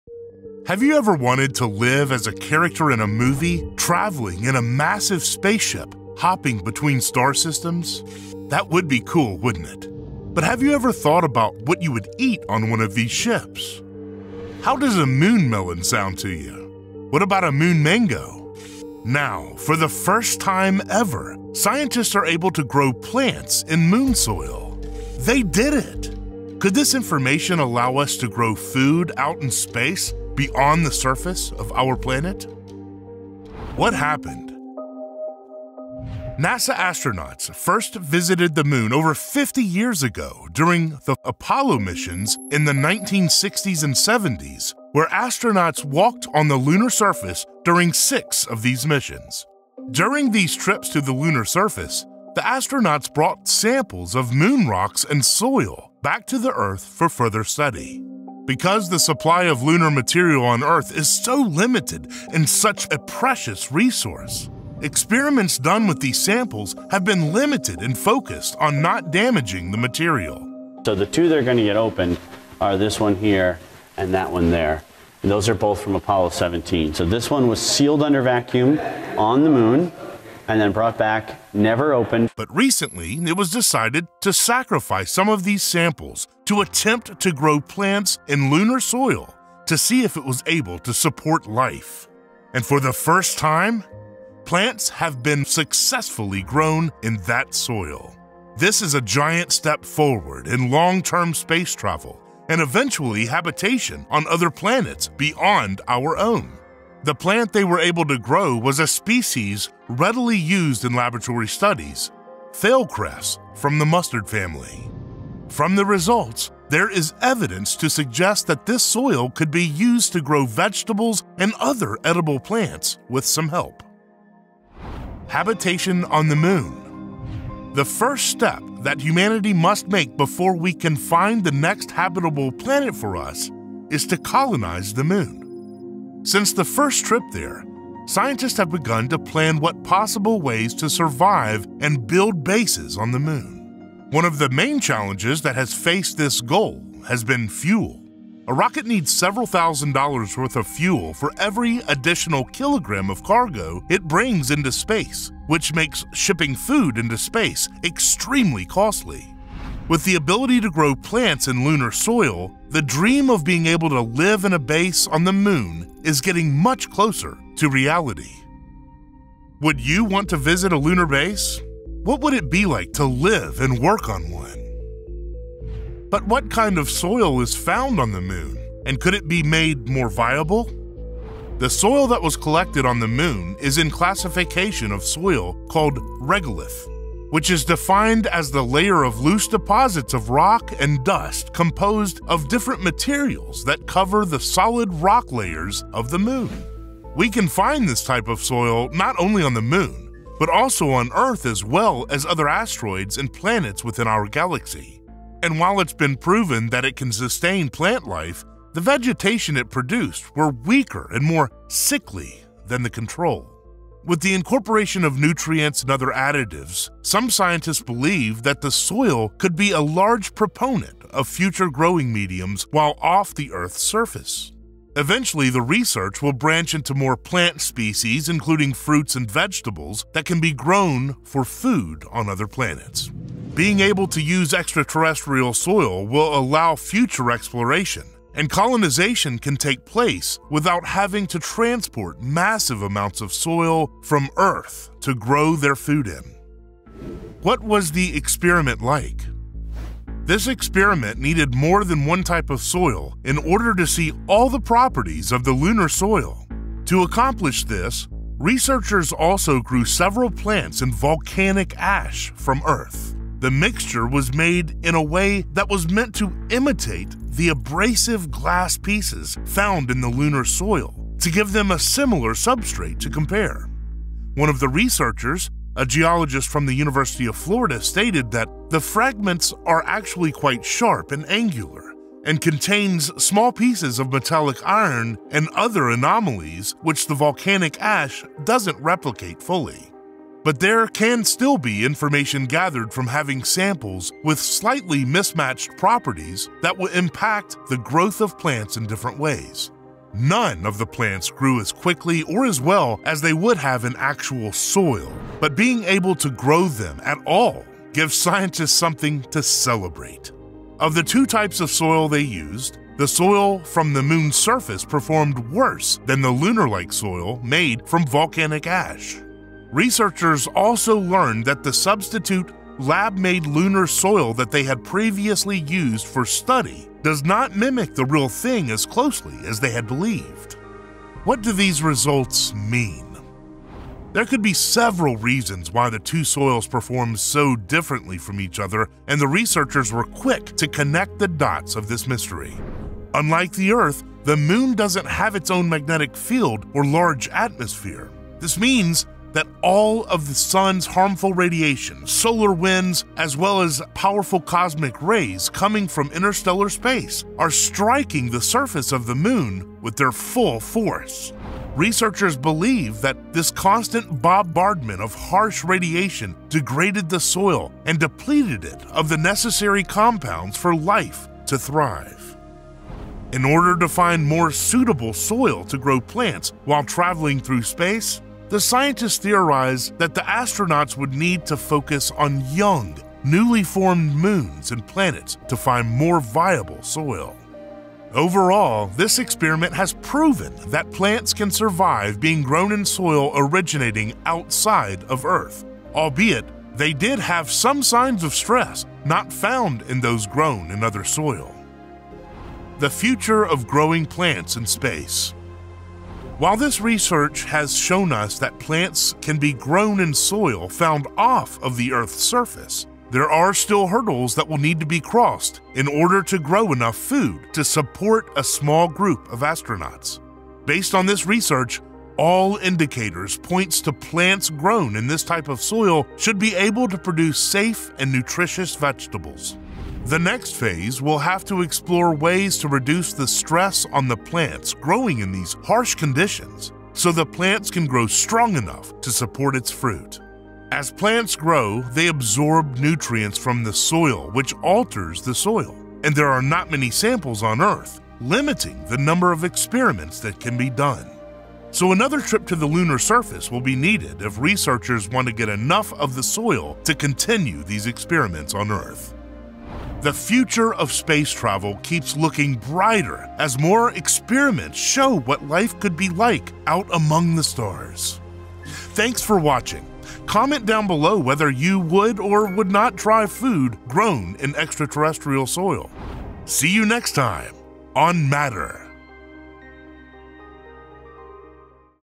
WE DID IT! Plants finally grown on Moon Soil
Growing plants in moon soil is some amazing recent space news that just came out. Moon farming can and will become something common in the future. As scientists continue to advance their technology, insane scientific breakthroughs just like this will change how we view space and the universe.

Topics Discussed/Related:
- Plants in moon soil news
- Plants in moon soil latest news
- Moon soil plant
- Plants in moon soil Drishti Ias